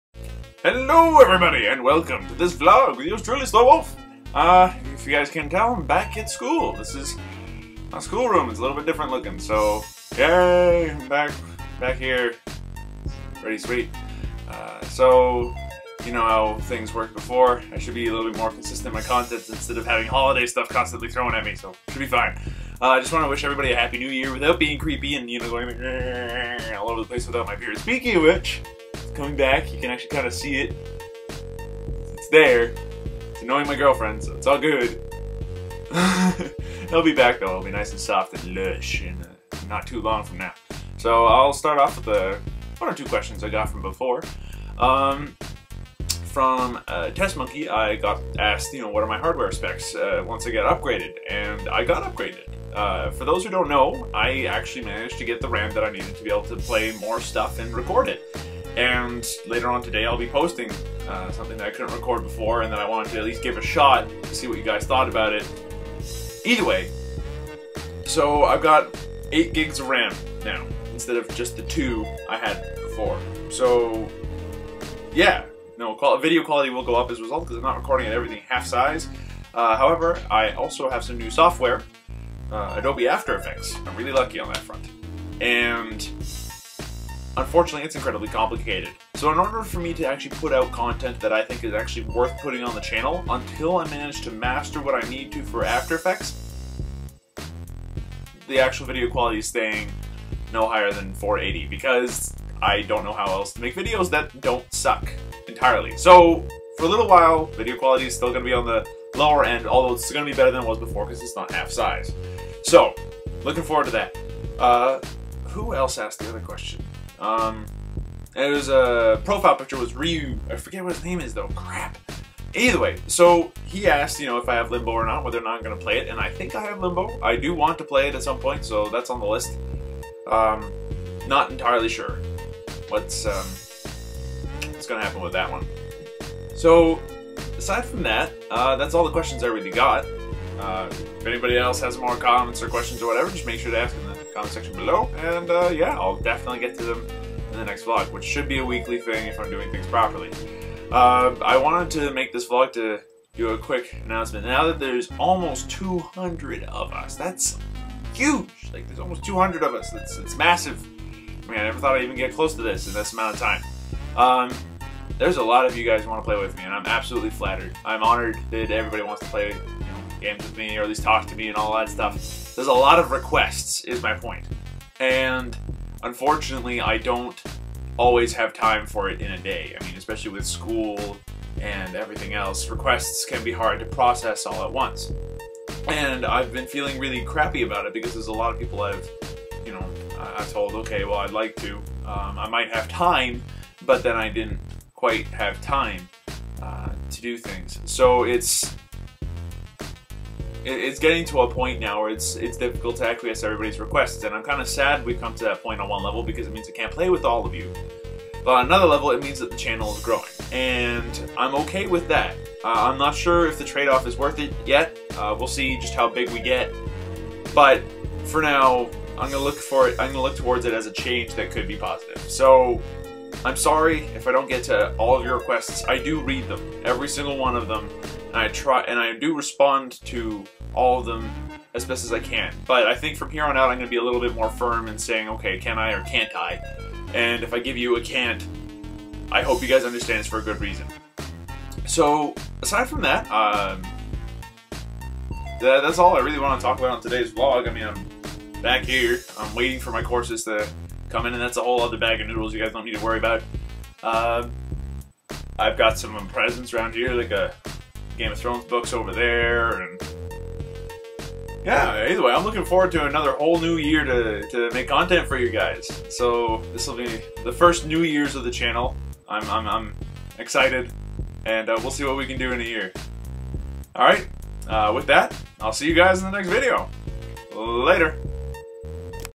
SlowWolf Vlog: Return to school, and a time of change
Hey guys! This vlog is just to show that I'm back at school, the return of the beard, and an announcement: we're getting bigger, and it's a time of change. Also, a proper Happy New year!

Thank you for liking, subbing, sharing, but above all watching!